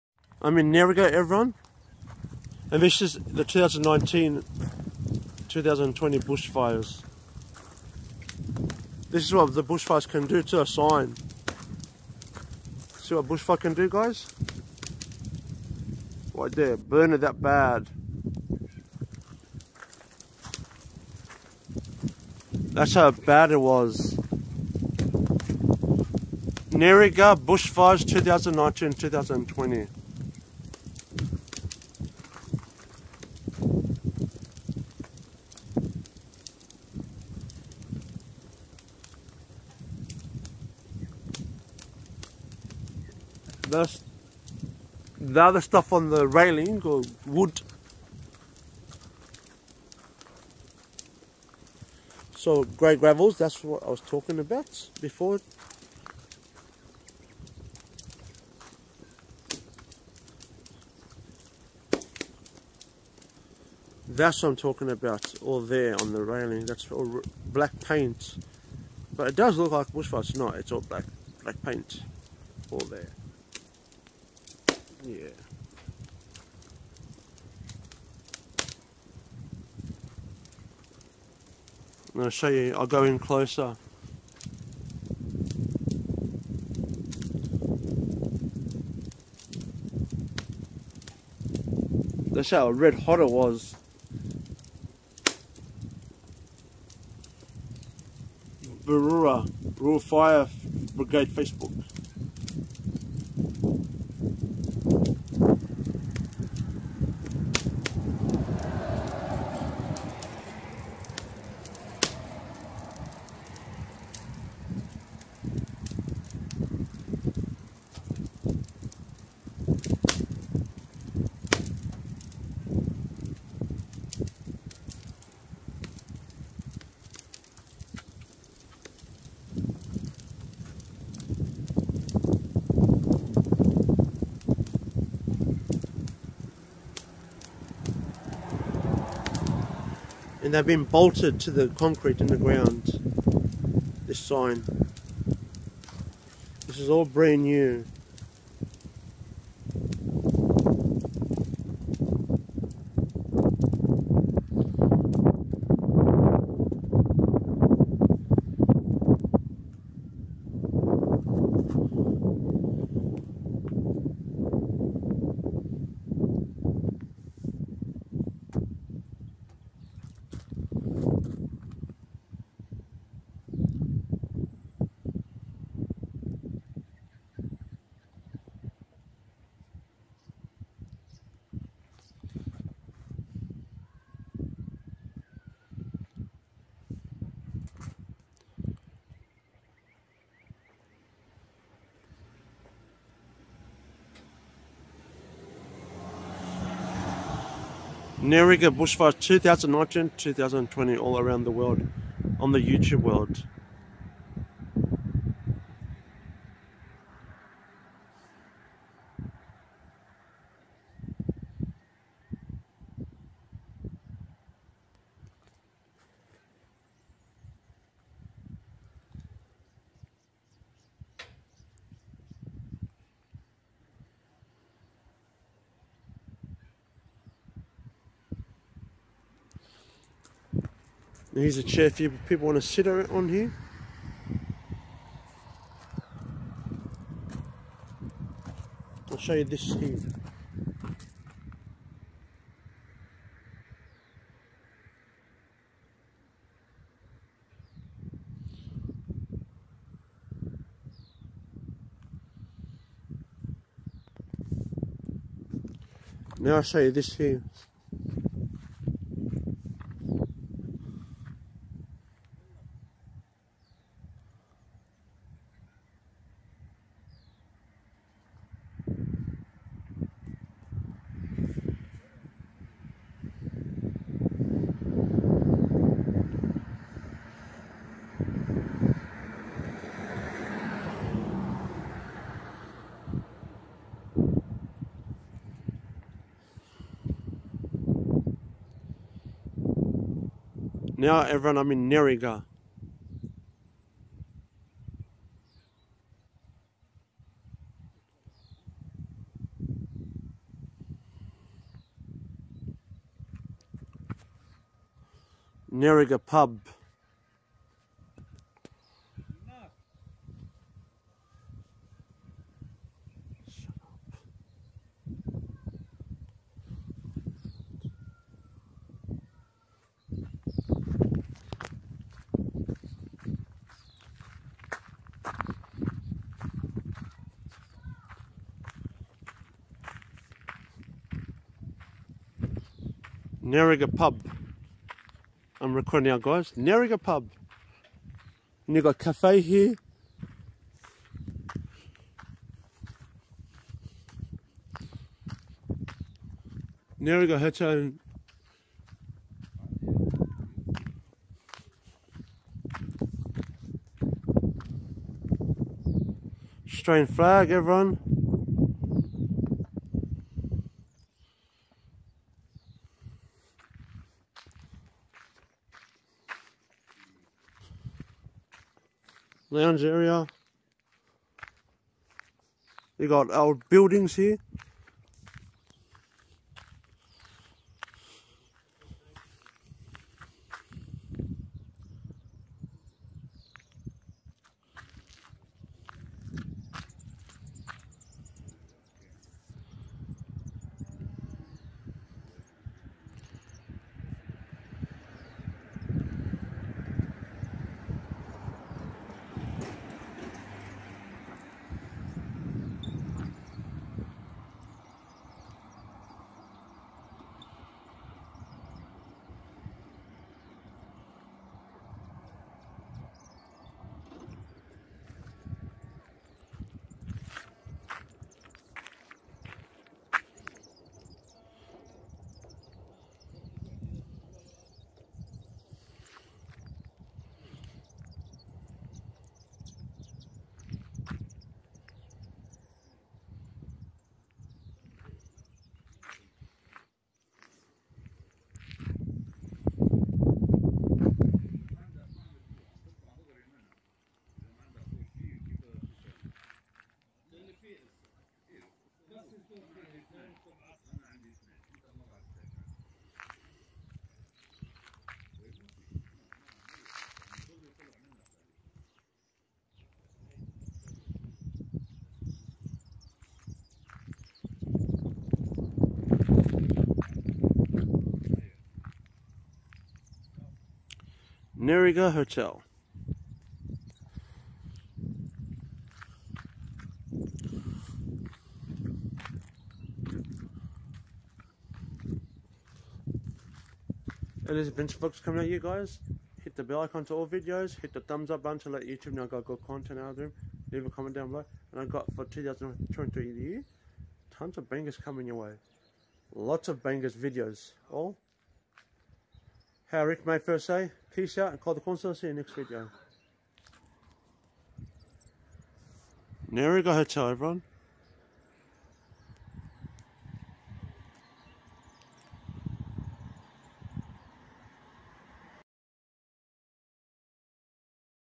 NerrigA Nsw Australia Pub
Established in the 1870s, the pub was originally known as the Commercial Hotel. It's located on the old Wool Road in the small village of Nerriga, which boomed following the discovery of gold in the area